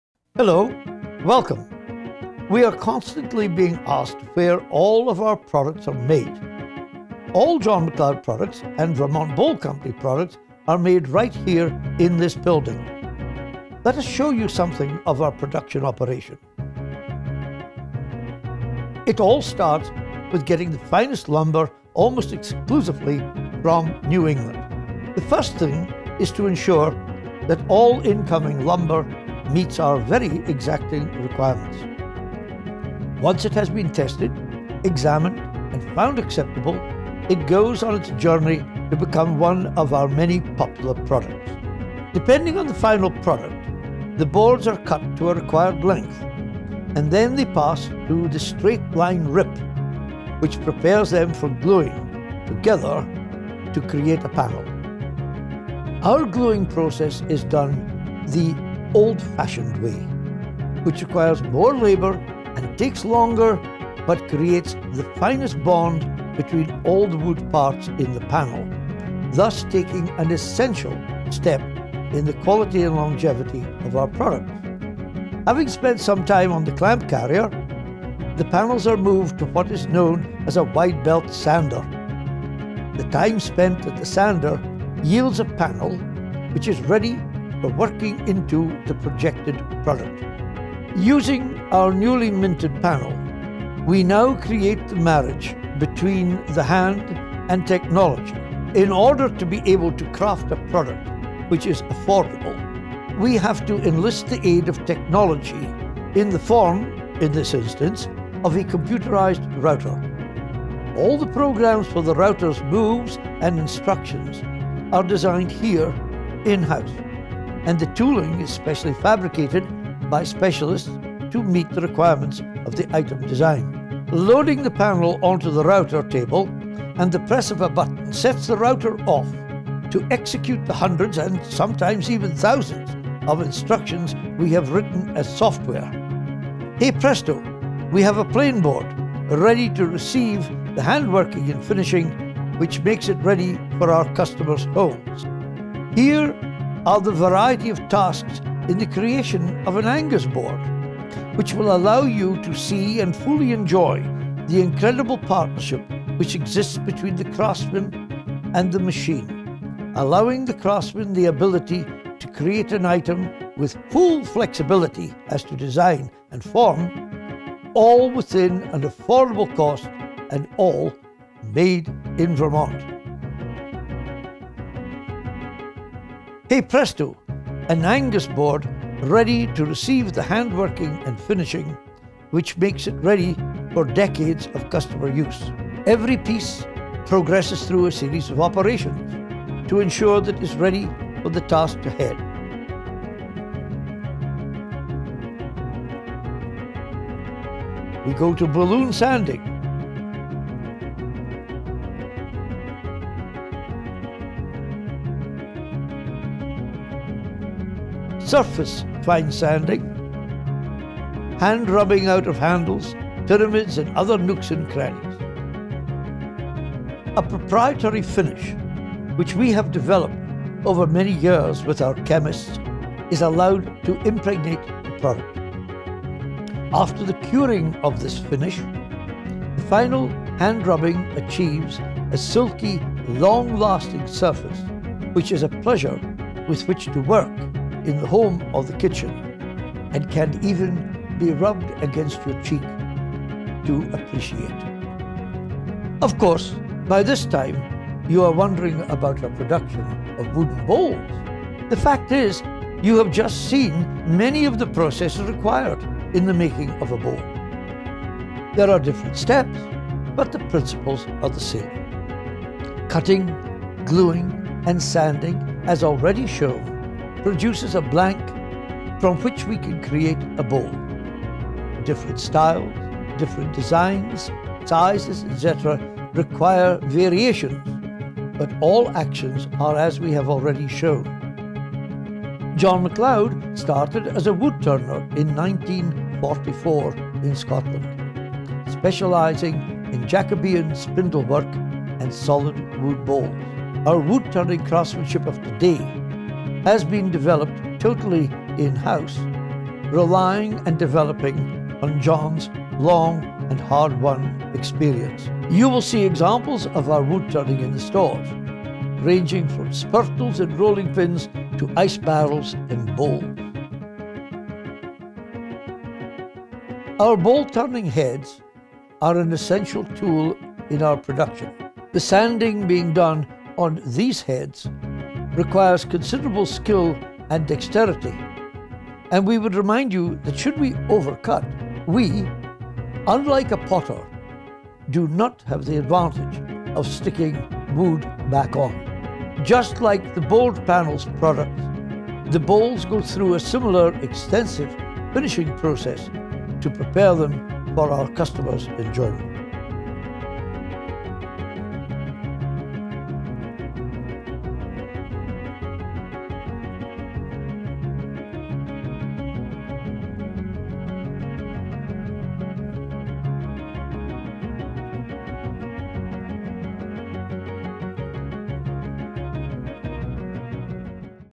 The Vermont Bowl Company Sneak Peek How It's Made
We are a manufacturer of wooden bowls, cutting boards, and much more. See some of the inner workings of our factory in this brief video recorded right here in Southern Vermont.